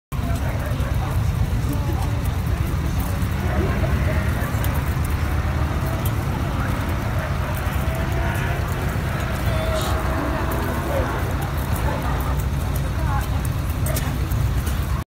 Хрущи атакуют уличный фонарь
На Валу в Чернигове